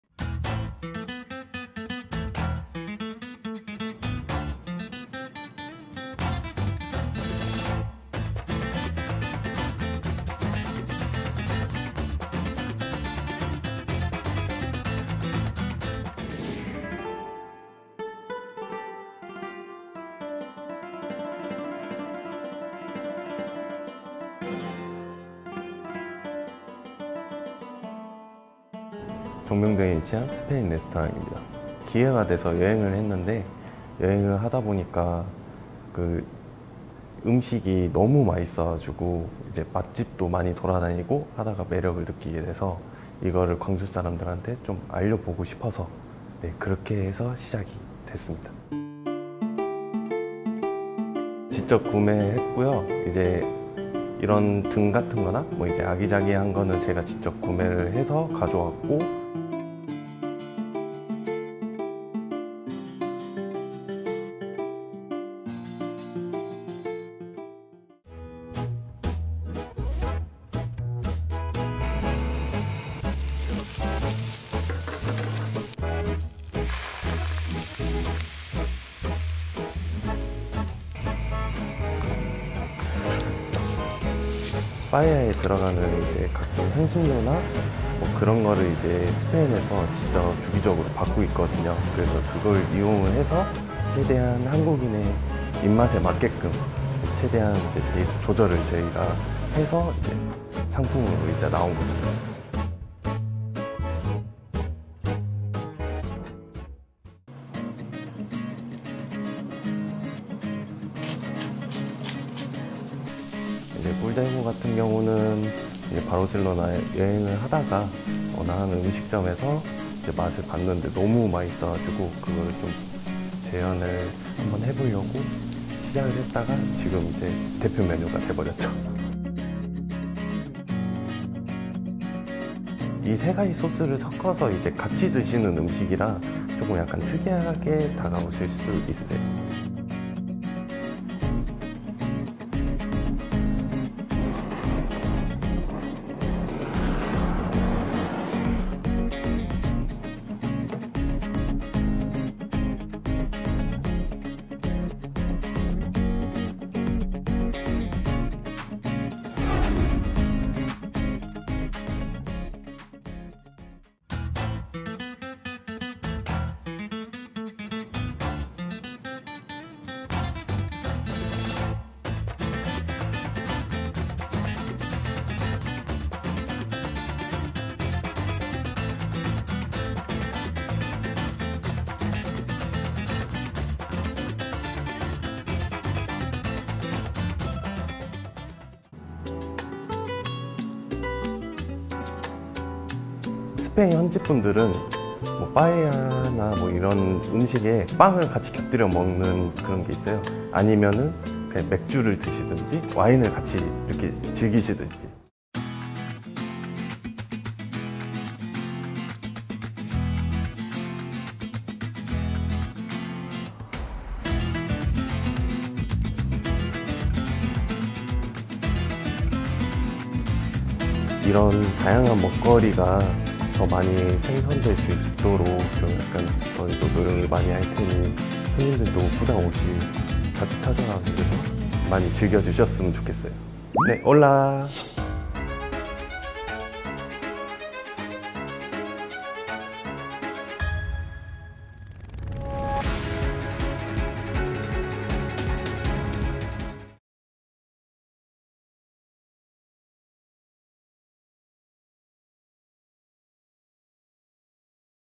스페인 현지 음식 다 훔쳐 옴! 스페인 요리 계의 문익점 ‘까라벨라데오로’ / 월간본슐랭가이드
‘스페인 요리’ 하면 떠오르는
빠에야, 감바스부터
한국에서 찾아보기 어려운 꿀대구까지!
광주 스페인 요리 맛집을 소개합니다

🇪🇸 까라벨라데오로
광주 동구 동계천로 131 2층